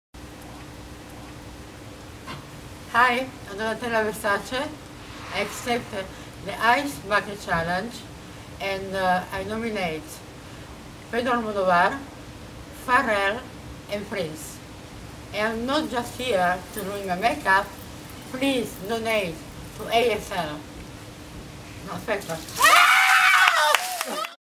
Donatella Versace ALS Ice Bucket Challenge
Donatella Versace does the Ice Bucket Challenge for ALS